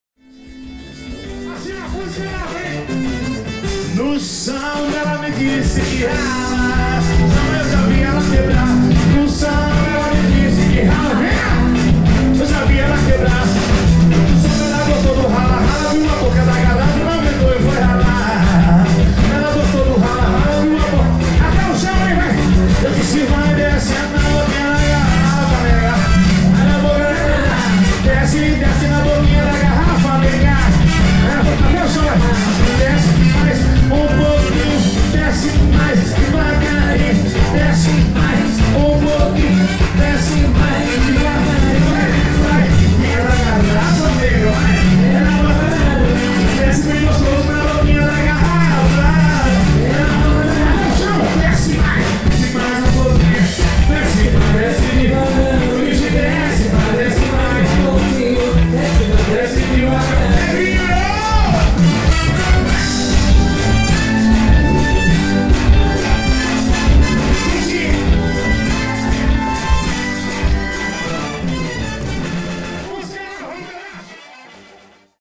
BANDA BAILE BH BOCA DA GARRAFA CARNAVAFAVELA 2015
Gravado ao Vivo CARNAFAVELA 2015 BARRAGEM SANTA LUCIA BH MG CARNAVAL EM BH CIA DUBHAYLE A BANDA MAIS ANIMADA DE BH CARNAVAL RÉVEILLON SHOWS E EVENTOS